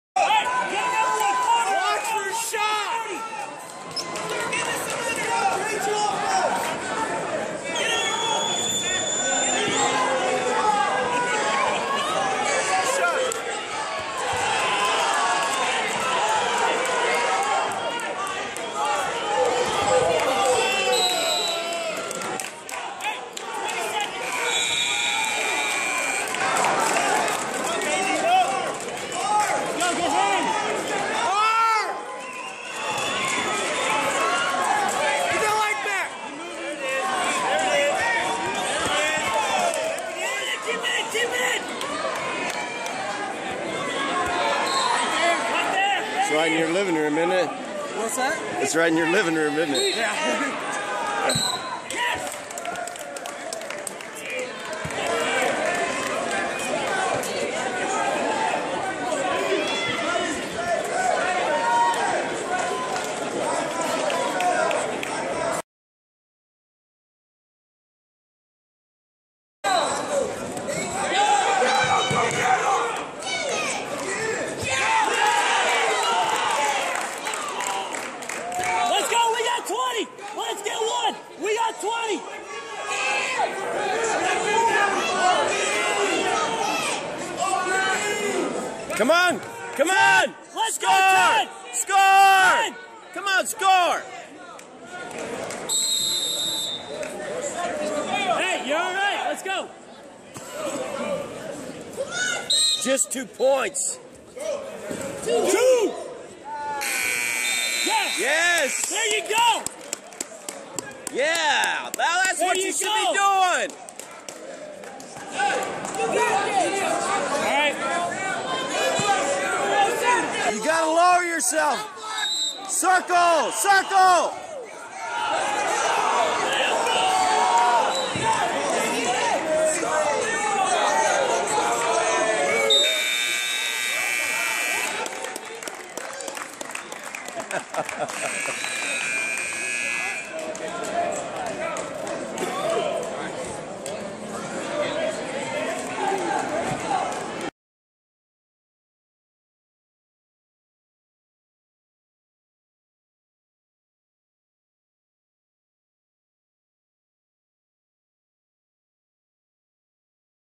Anthony Davis at 2016 Mahomet Sectionals
Central Maroon Wrestler Anthony Davis at 285# Class